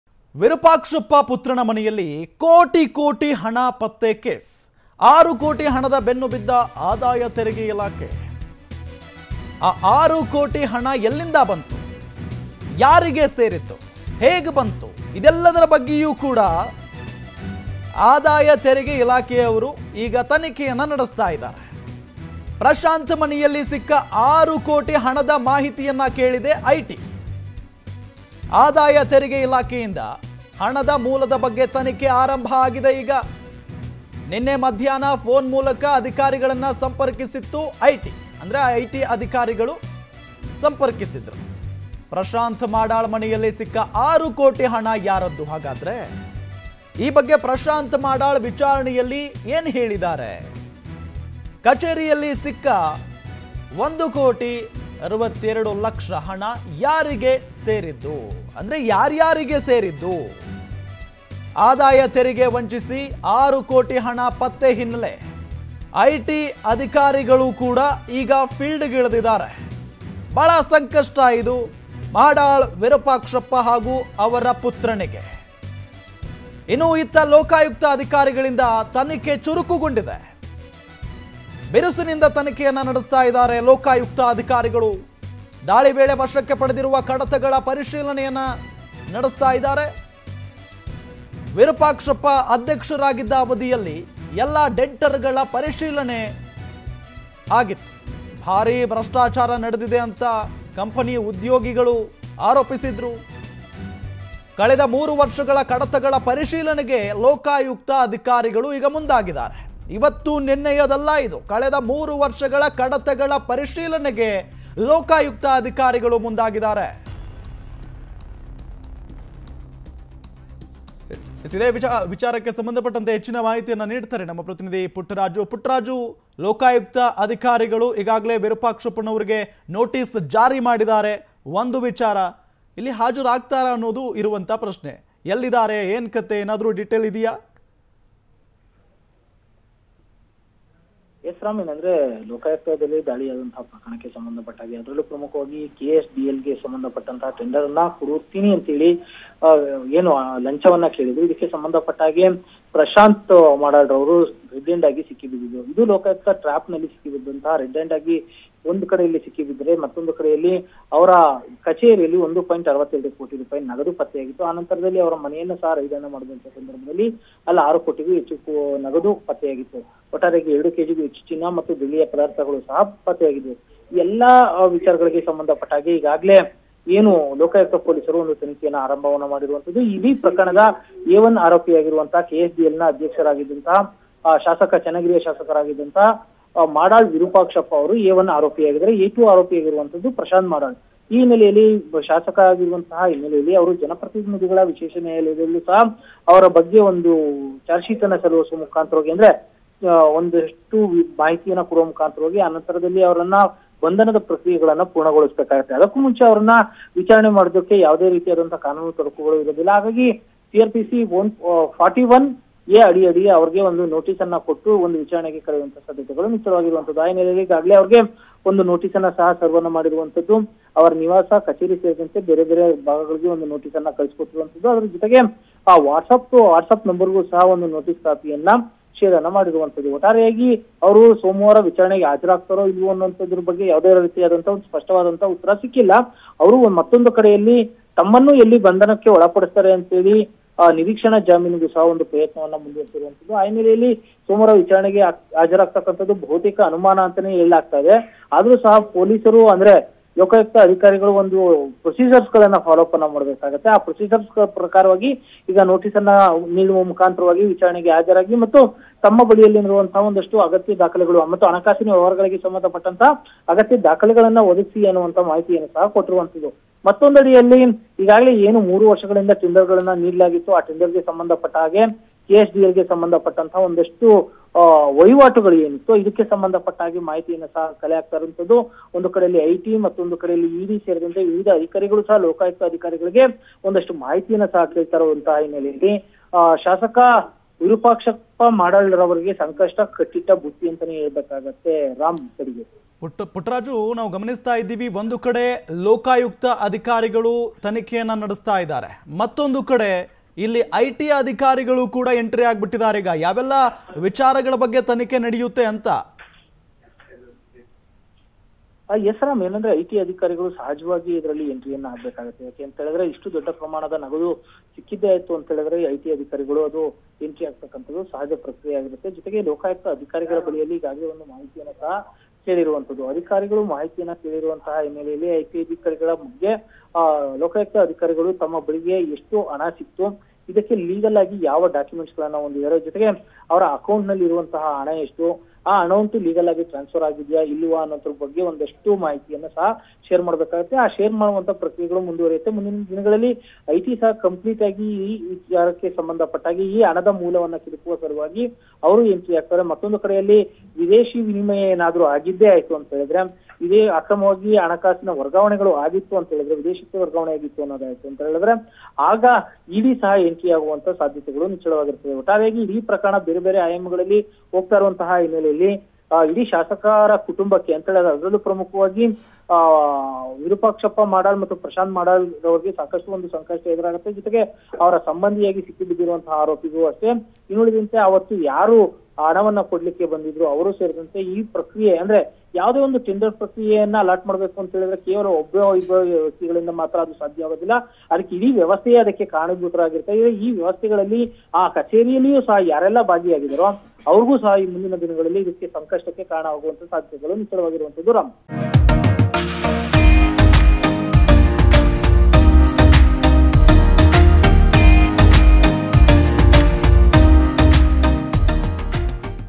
Prashanth Madal : ಪ್ರಶಾಂತ್ ಮಾಡಾಳ್‌ ಮನೆಯಲ್ಲಿ ಸಿಕ್ಕ 6 ಕೋಟಿ ಹಣ ಯಾರದ್ದು..? | Power TV News
More Updates Keep Following Us